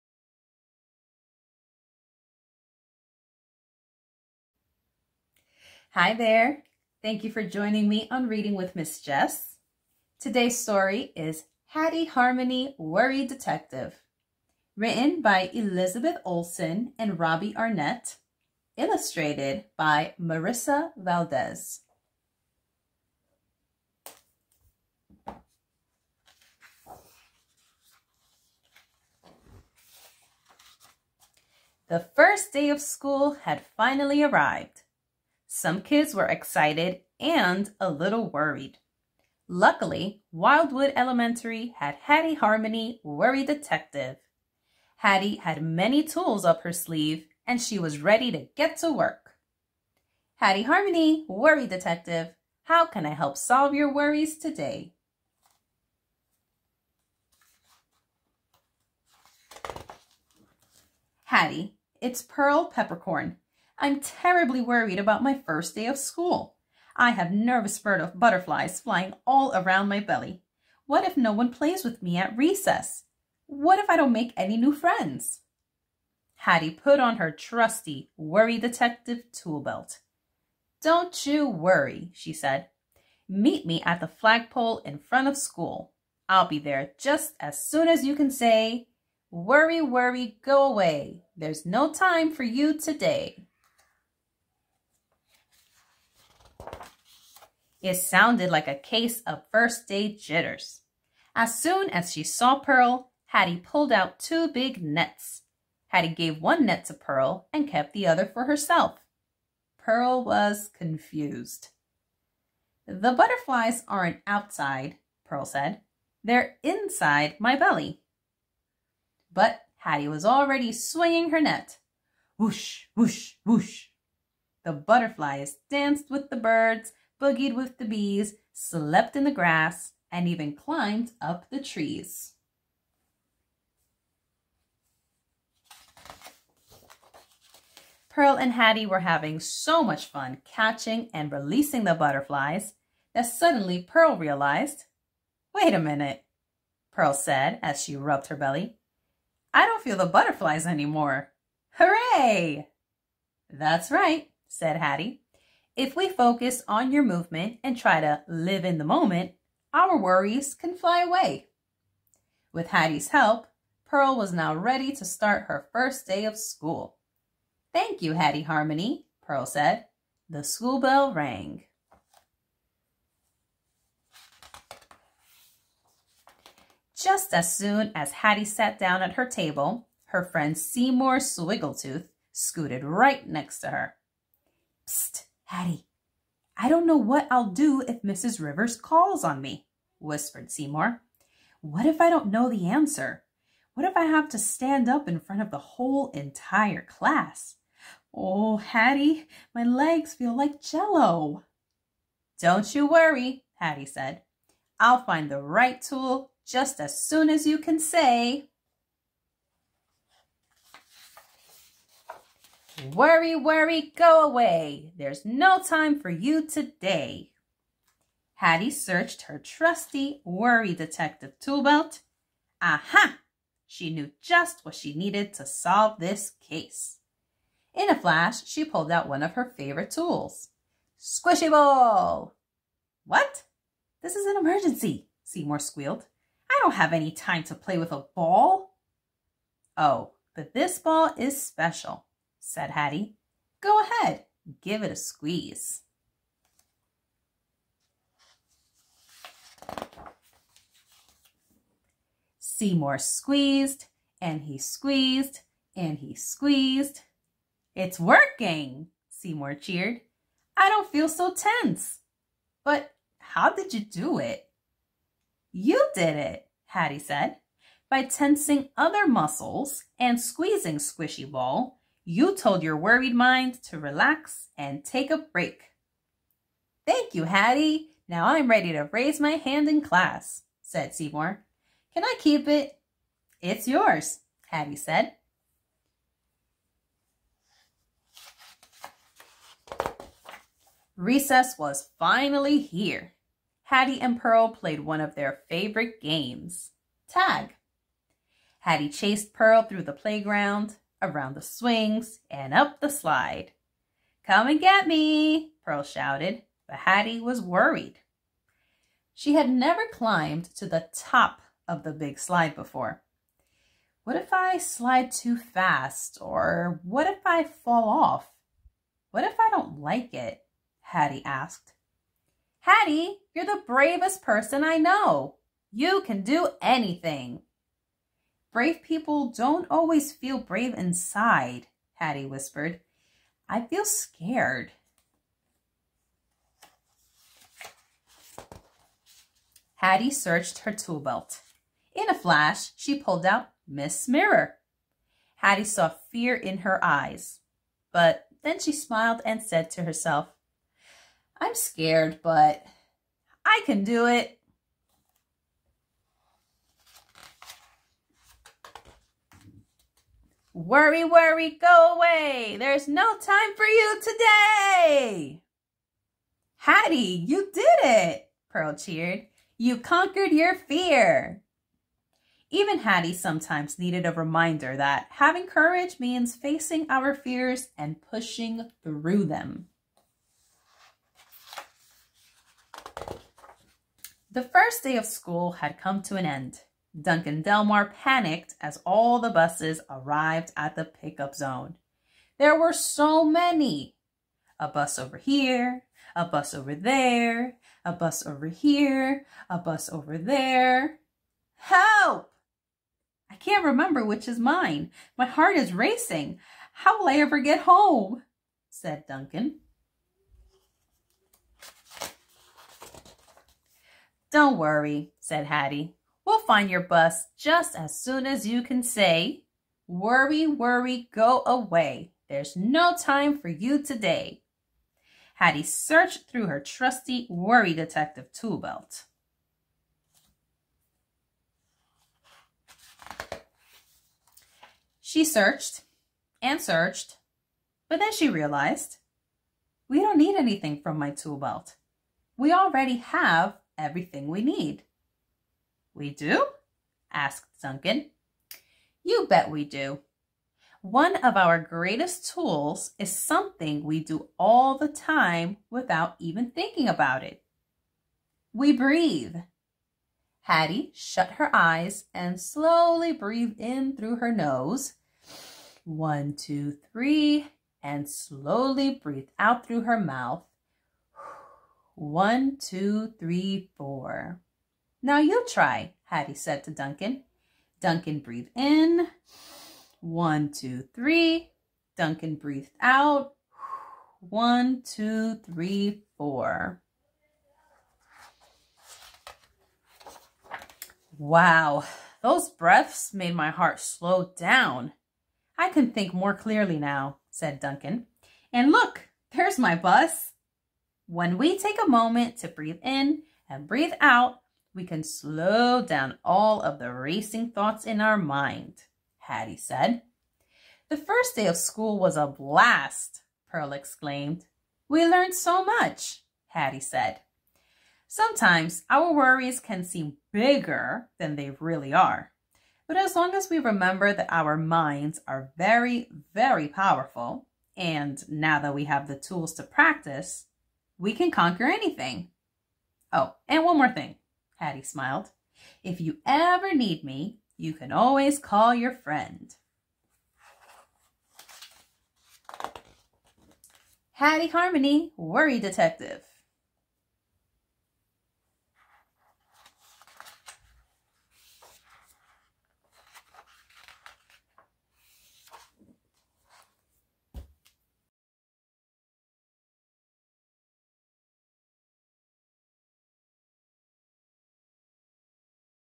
Hattie Harmony: Worry Detective Read Aloud by Ms. Yes
Ms. Yes reads Hattie Harmony: Worry Detective written by Elizabeth Olsen and Robbie Arnett and illustrated by Marissa Valdez

“Award-winning actress Elizabeth Olsen and esteemed musician Robbie Arnett have created a fresh approach to wellness in a new series that shows children ways to manage anxiety. A perfect back-to-school read! Meet Hattie Harmony, Worry Detective. She's always there to help her friends when they need her. And the first day of school can cause a lot of jitters--from speaking in front of the class to taking the bus. Hattie always finds the perfect tools to calm her friends' bodies and minds. But when her own fears start to bubble up, can Hattie learn to overcome them herself? Critically acclaimed actress Elizabeth Olsen and esteemed musician and writer Robbie Arnett have created an irresistible new character filled with warmth and humor to introduce the youngest readers to the importance of self-care.” - thriftbooks